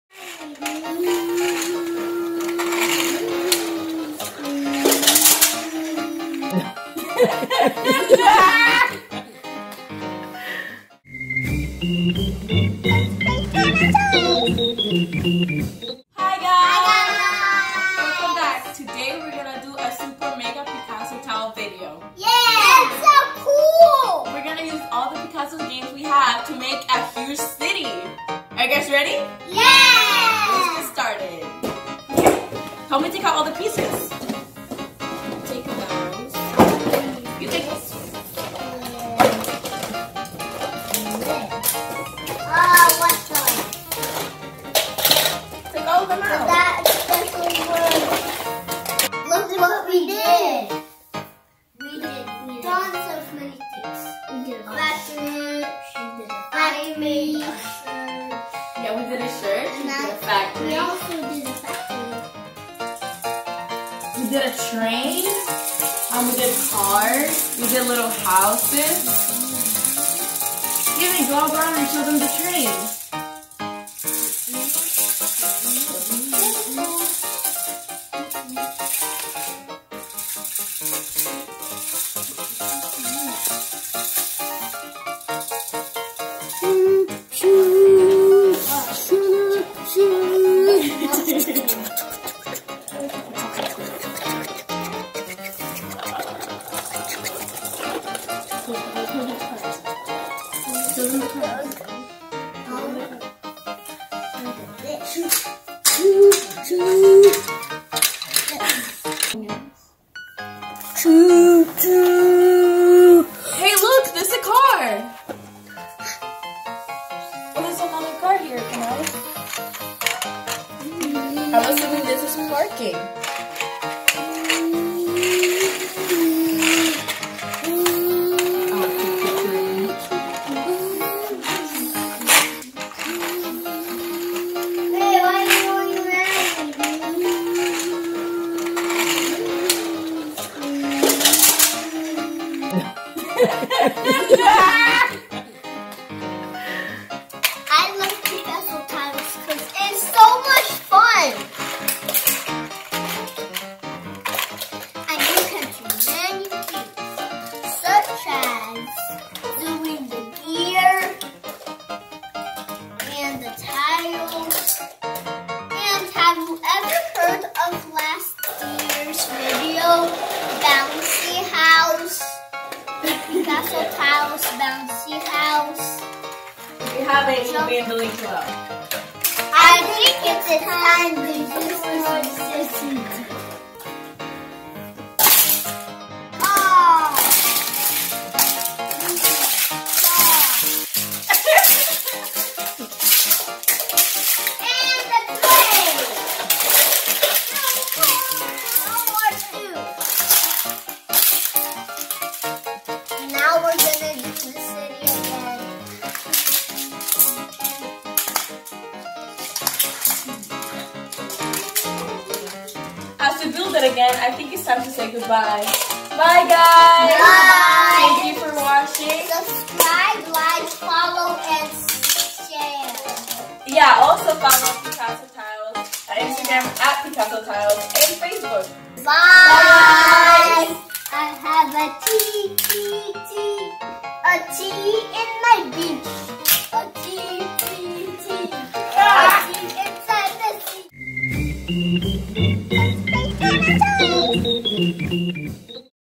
Picasso Tiles City Playground 4 Magnetic Sets and 2 Twins
Let US show you how much Fun you can have with Picasso Tiles, this magnetic blocks are so much Fun.  We combined 4 complete sets for this video and created a Mega City playground.

PTN100 Nuts and Bolts Building Block

Amazon:PT180 magnetic tile set delux